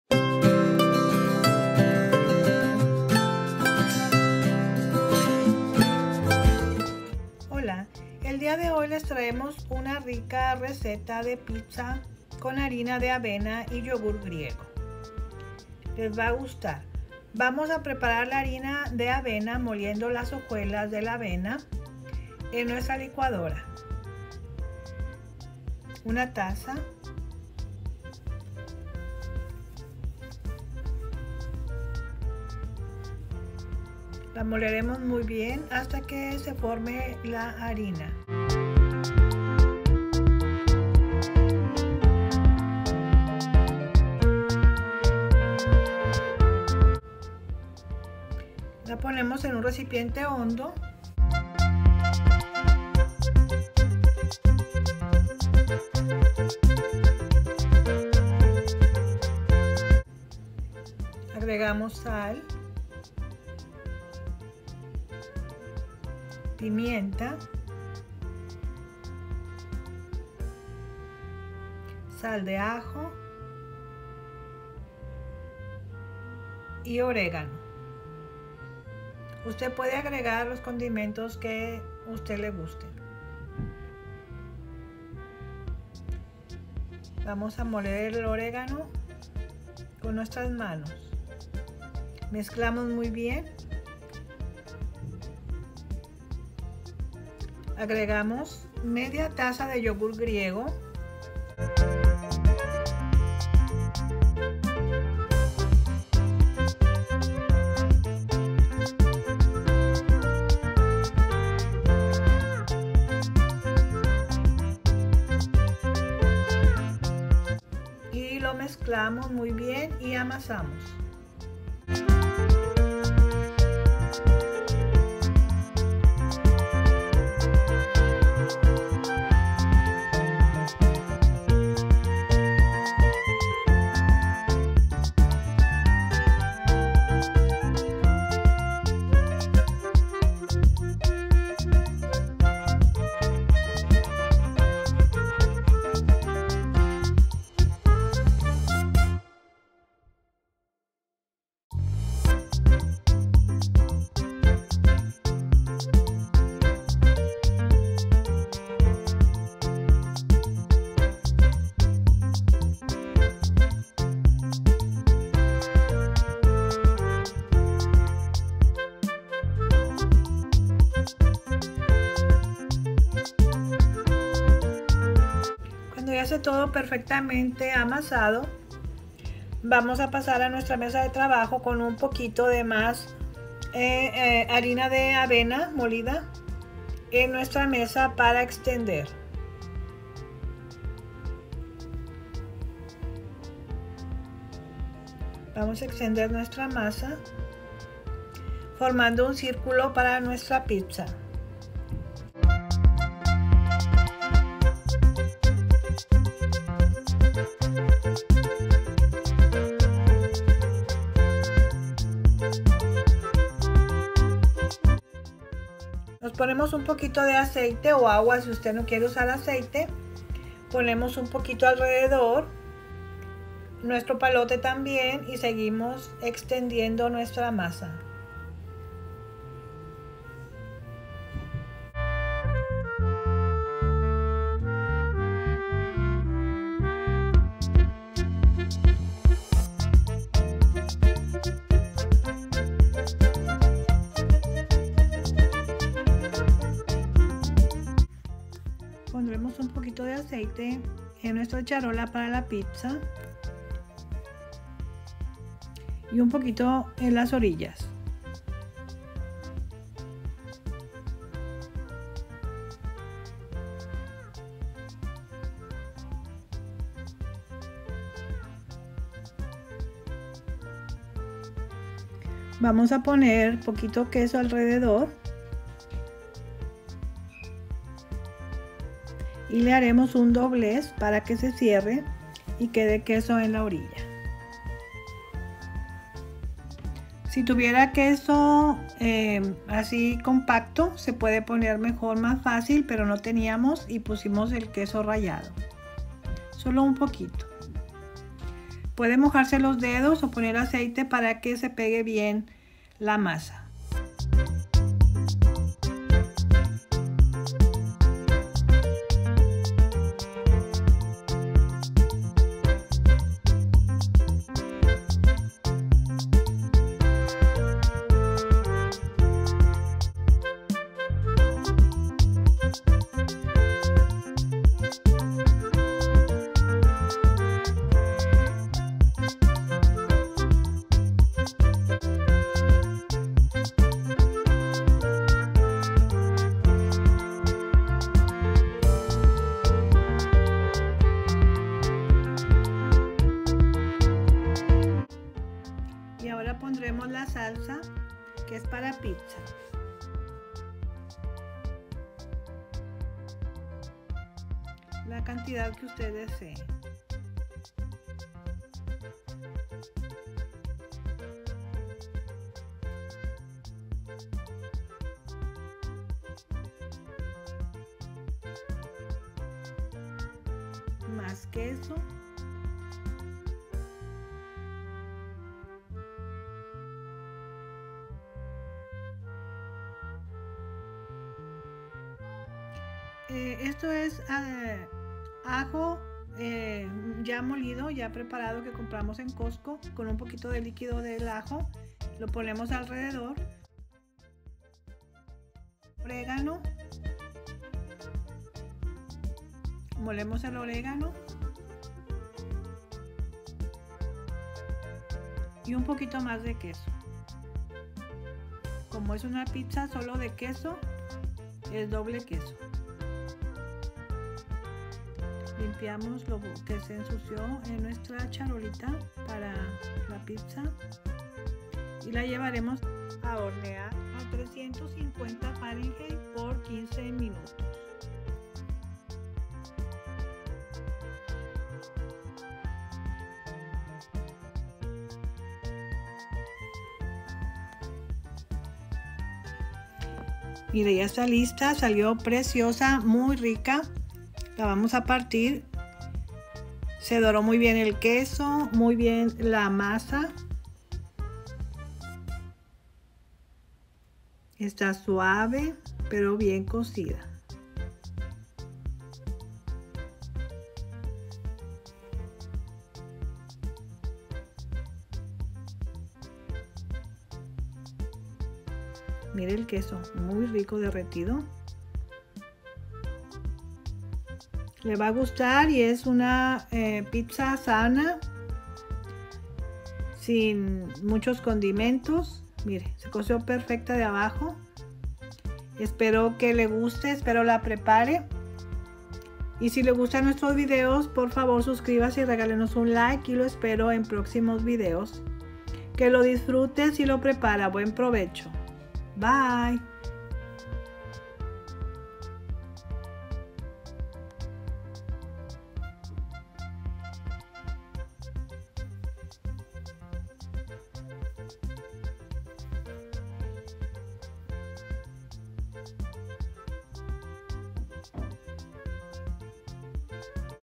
COMO PREPARAR PIZZA 🍕 CON HARINA DE AVENA😱 y yogurtgriego CON ORILLA DE QUESO 🧀!!
HOLA AMIGOS HOY PREPARAMOS PIZZA 🍕
CON HARINA DE AVENA Y YOGURT GRIEGO. EXQUISITO

INGREDIENTES:
1 TZA. DE HOJUELAS DE AVENA
1/2 TZA. DE YOGURT GRIEGO
QUESO MOZZARELLA
SAL AL GUSTO
PIMIENTA, SAL DE AJO, OREGANO AL GUSTO
UN POQUITO DE AJO PICADO PARA LA ORILLA DE LA PIZZA (OPCIONAL)
SALSA PARA PIZZA DE SU PREFERENCIA (NOSOTROS USAMOS PREGO)

HORNEAR SEGÚN EL VÍDEO